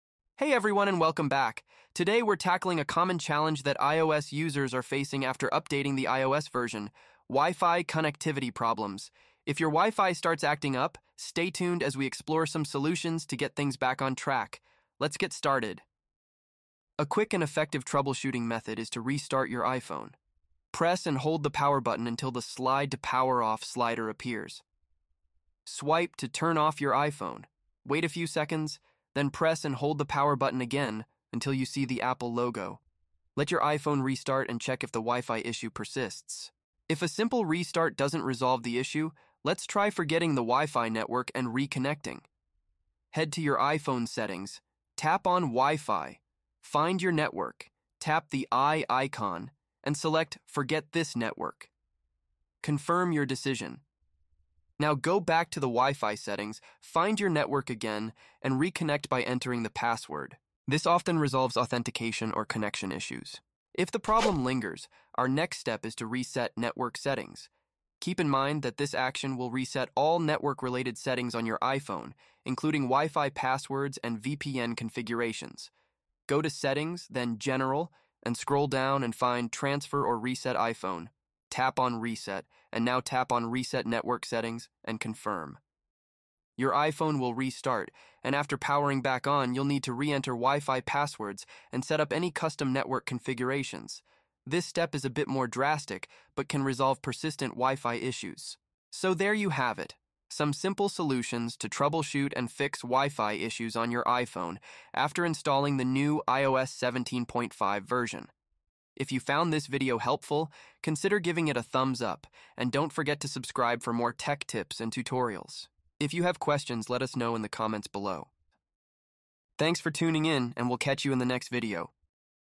iOS 17.5 - Fix WiFi Issues After Updating!
Learn How To Fix WiFi Issue after installing iOS 17.5
Are you facing WiFi connectivity problems after updating to iOS 17.5? In this video, we've got you covered with a comprehensive troubleshooting guide to resolve WiFi issues on your iPhone or iPad. From basic steps to advanced techniques, learn how to tackle common WiFi problems effectively. Watch now and say goodbye to WiFi woes with our easy-to-follow solutions!